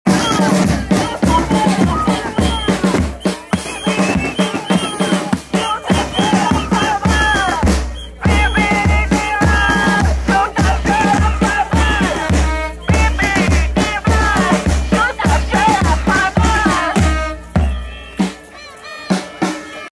חיפה: חסימת צומת חורב והפגנה במחאה על פיטורי גלנט | צפו
קרדיט: מחאת העם חיפה